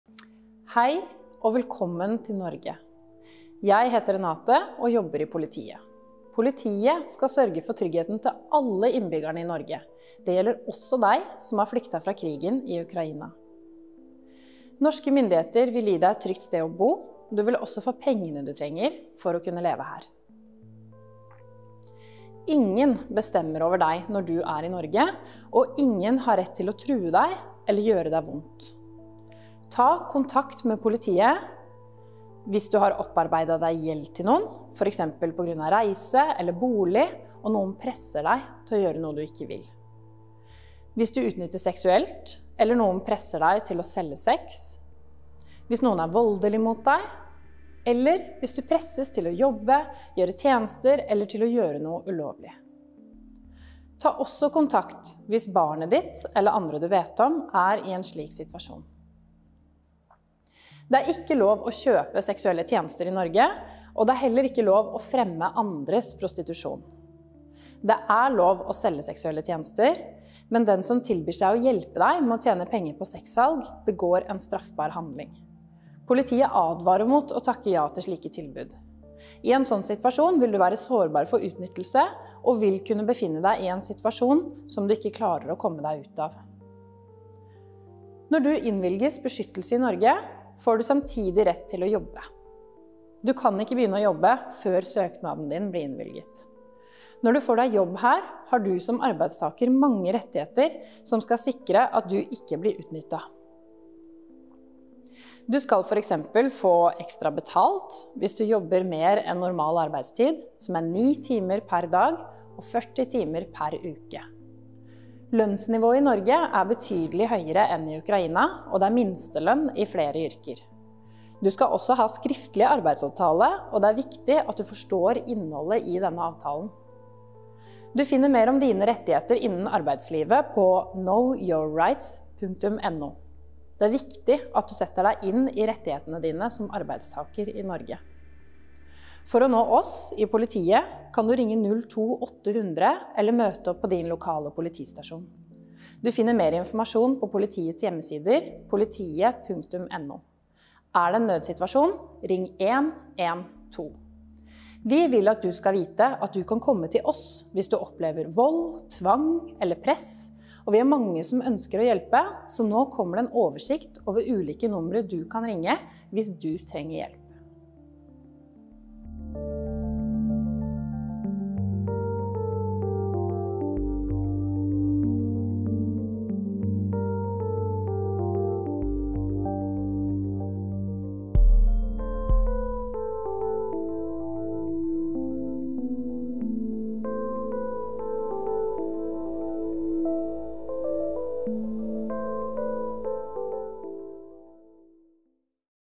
Приветствуем вас в Норвегии!
En informasjonsvideo til ukrainske flyktninger fra politiet. Videoen er tekstet til russisk.